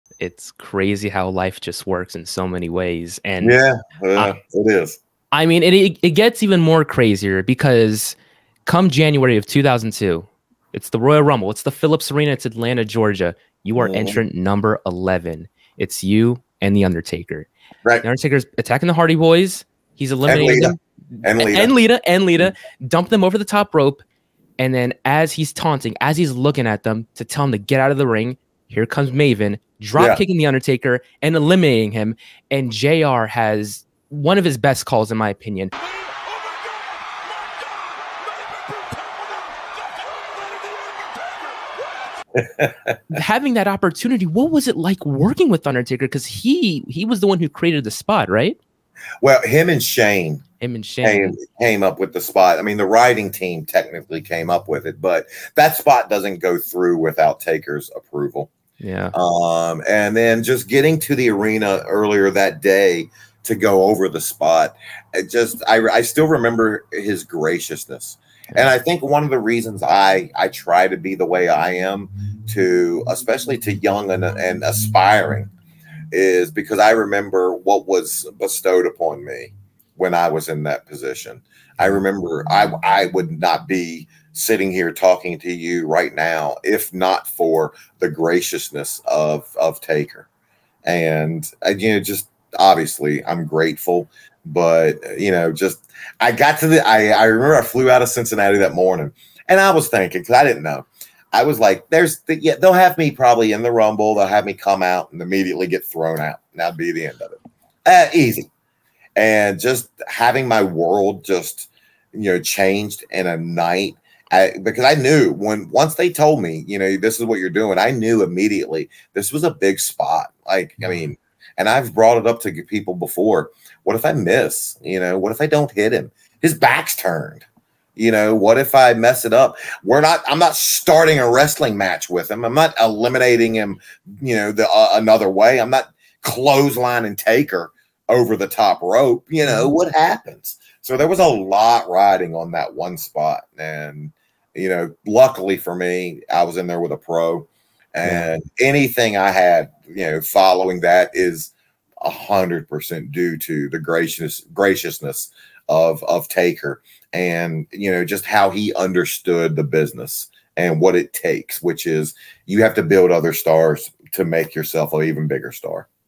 "There Was A Lot Riding On That One Spot" | Maven On Eliminating Undertaker From The Royal Rumble
Former WWE Superstar, Maven (@MavenKHuffman) shares his insight on eliminating The Undertaker during the 2002 Royal Rumble Match.

Maven Huffman is an American professional wrestler, YouTuber, and media personality. He is best known for his time with WWE, where he is known for co-winning the inaugural season of Tough Enough alongside Nidia Guenard. He also became a three-time WWE Hardcore Champion.